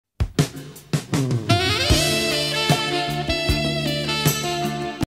Majestic Mountain Gorilla in Lush Green Mountains | Digital Artwork.
Immerse yourself in the beauty of this stunning digital artwork featuring a majestic mountain gorilla in a lush green mountains.